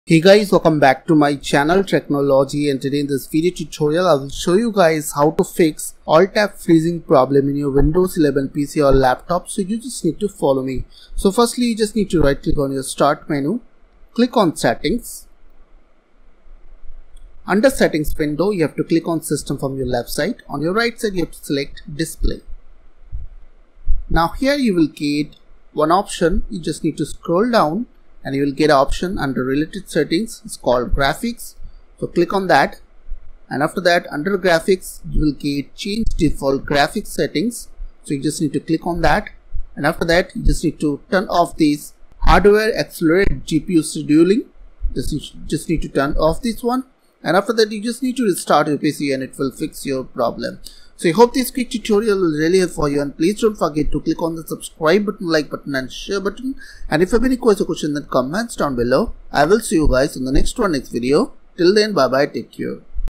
How To Fix Alt+Tab Freezing in Windows 11[Solution]
Here in this video tutorial, I will show you guys how to fix Windows 11 crashing after pressing the alt-tab.